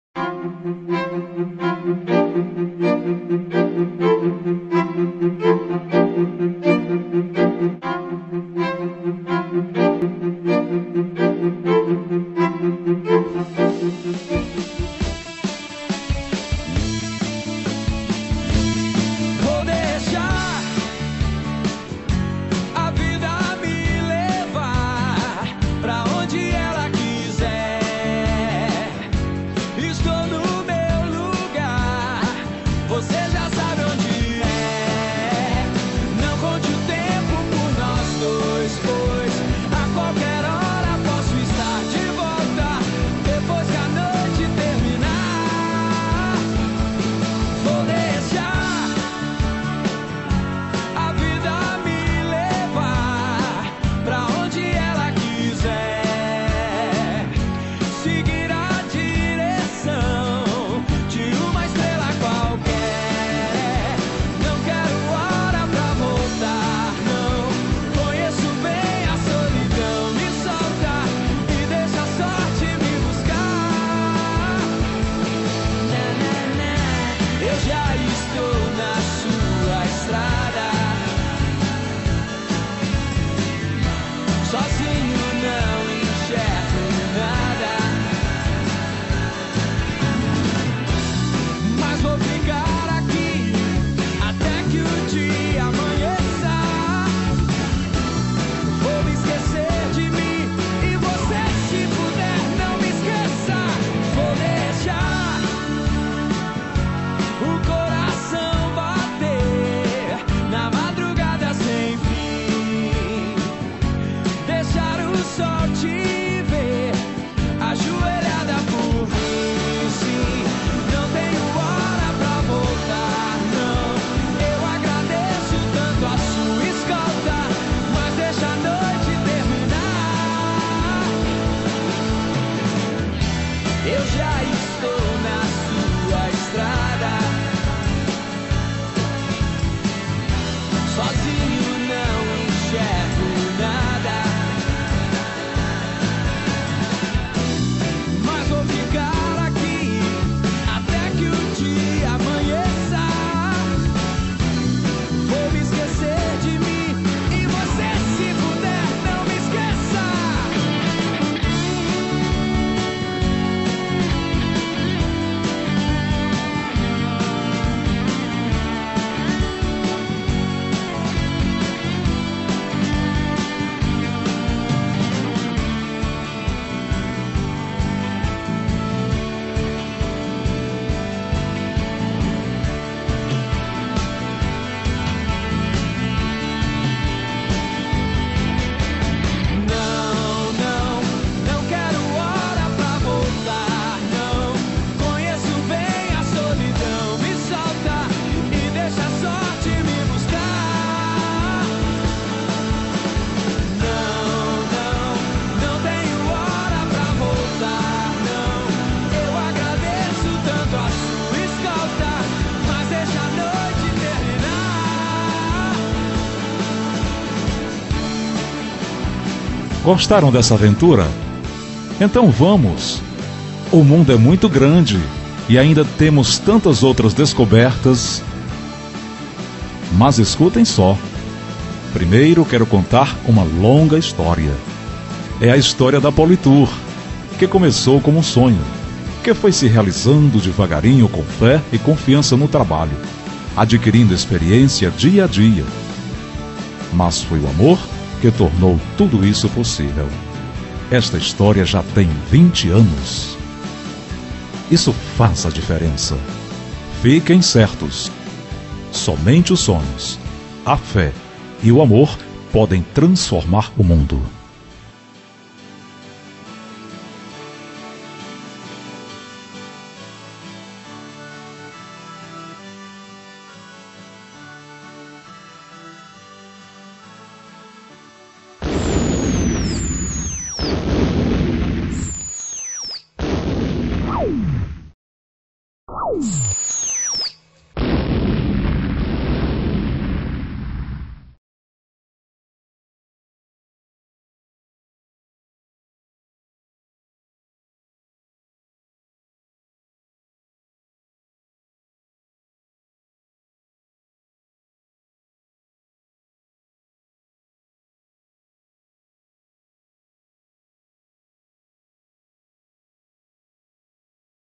Institucional Turismo
Video Institucional realizado em comemoração de 20 anos de uma empresa de turismo. editado por Davi FS.